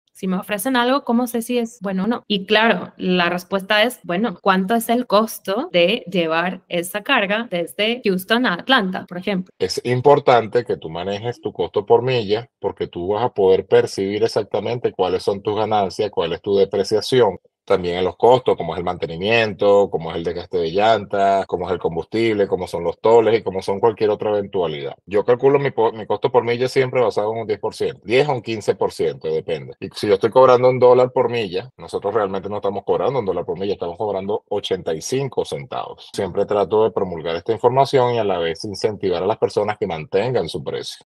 ¡Nos lanzamos con todo en el taller en vivo de Bobtail junto a @loboencarretera! 🚛💡
Aprendimos a calcular el costo por milla, recibiendo tips geniales de la mano de Luis Luvie. 📊🔧

¡Gracias a todos los que asistieron, sumando conocimiento y mejorando en el negocio!